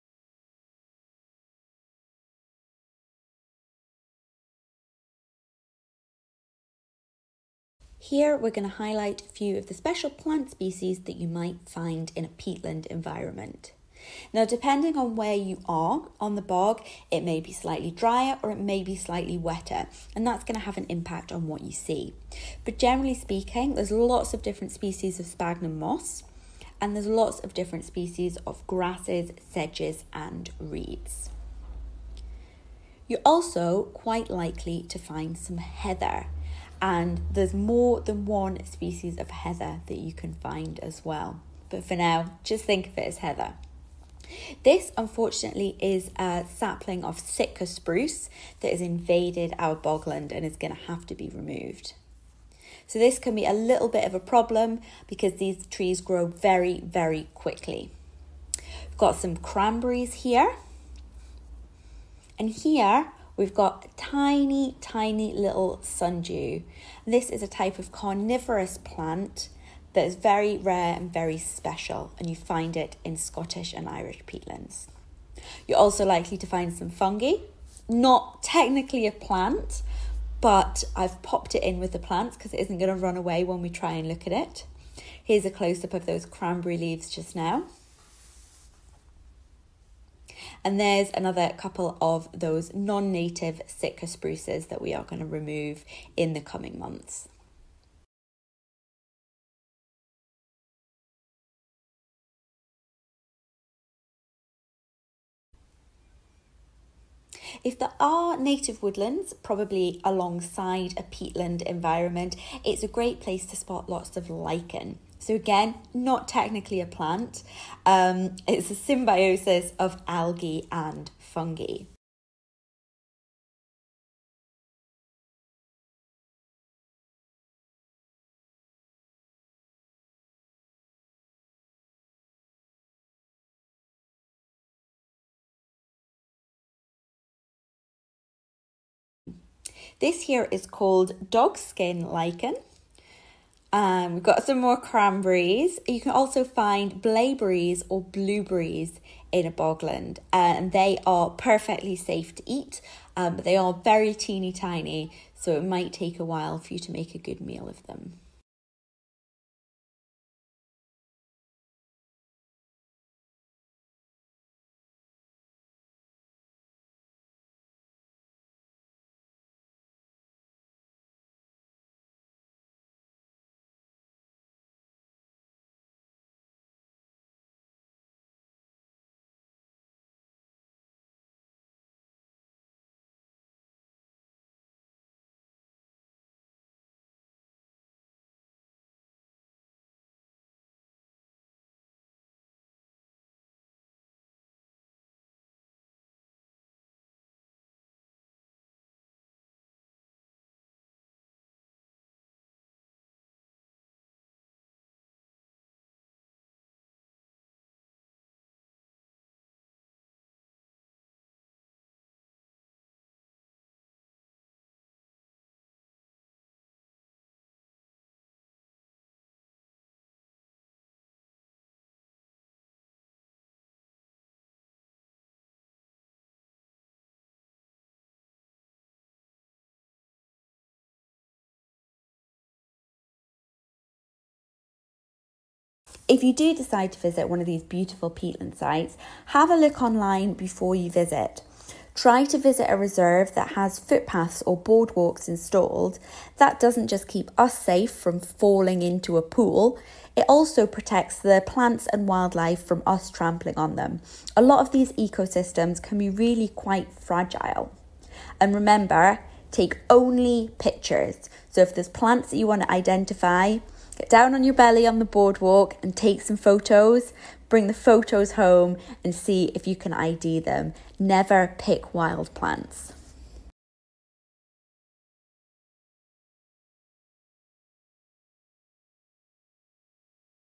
Peatland Plants | Laura Curtis-Moss, East Ayrshire Coalfield Environment Initiative
Peatland Plants with East Ayrshire Coalfield Environment Initiative

As part of Science on the Sofa's online programme, Laura Curtis-Moss highlights just a few of the plants you might spot on a bog.